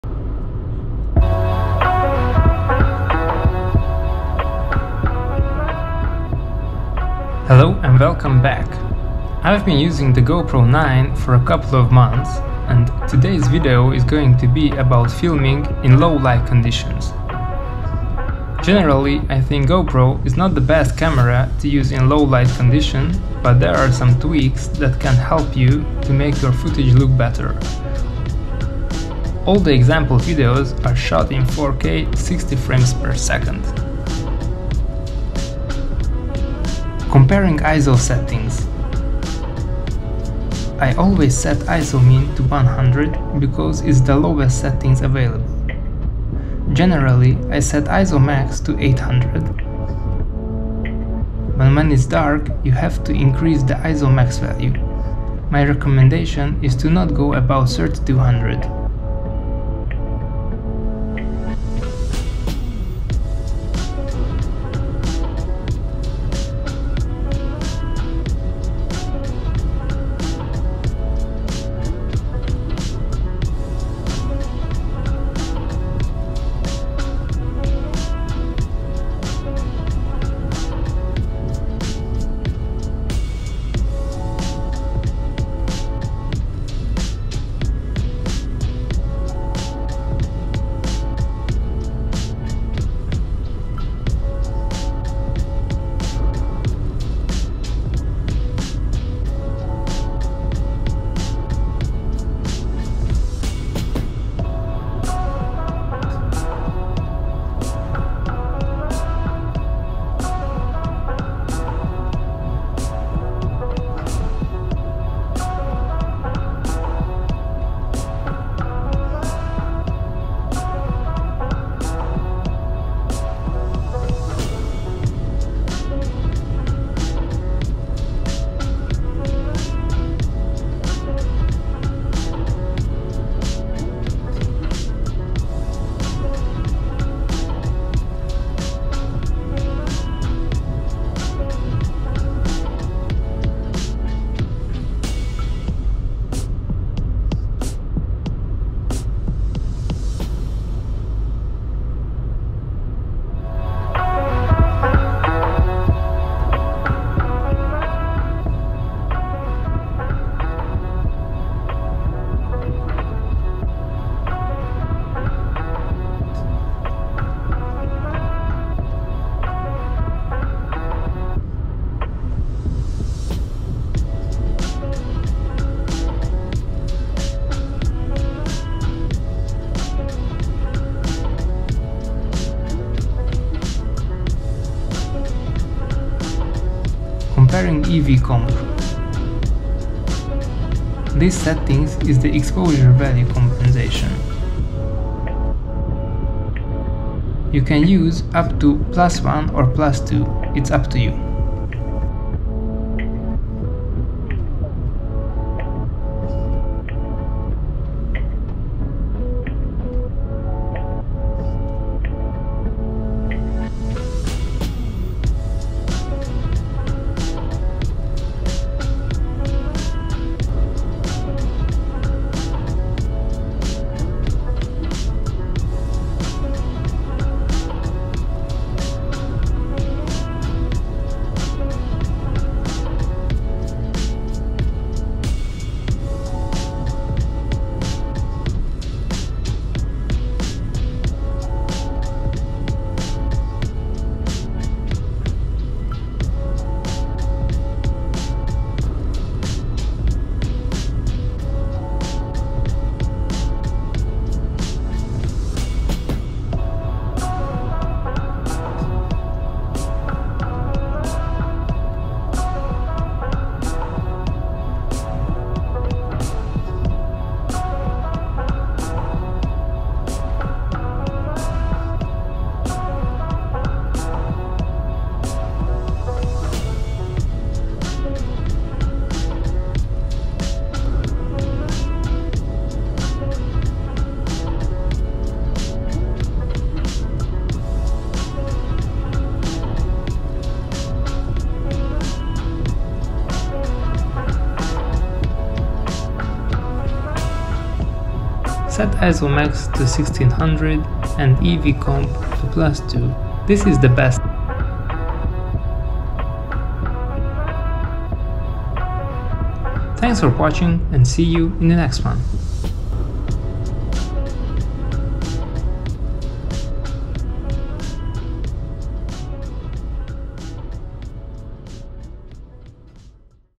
GoPro Hero 9 Black - BEST SETTINGS for NIGHT
GOPRO HERO 9 LOW LIGHT TEST- Watch Before You BUY!

GoPro has been around for many years now and they have had plenty of time to get it right. DJI released the Osmo Action in 2019 is ticked almost all the boxes in their first attempt.
Can the NEW GOPRO HERO 9 beat the Osmo Action in LOW LIGHT, DYNAMIC RANGE and COLORS? In this video I do a little bit of a test and compare the footage.
Action cameras are notorious for average low light performance due to the smaller sensor and smaller pixel density and now the GoPro has a newer larger MP sensor, will this effect the low light performance?

In this GoPro low light condition test I will test various settings of ISO MAX settings also the proper settings of EV Comp.

All the footage shot in 4k 60fps

Chapters:
00:00 GoPro Dark Settings - Intro
00:36 Comparing ISO settings
01:03 ISO MAX 800
01:33 ISO MAX 1600
02:03 ISO MAX 3200
02:34 ISO MAX 6400
03:06 Comparing EV Comp settings
03:30 EV Comp +1.0
03:48 EV Comp +2.0
04:19 Best Settings: ISO MAX 1600, EV Comp +2.0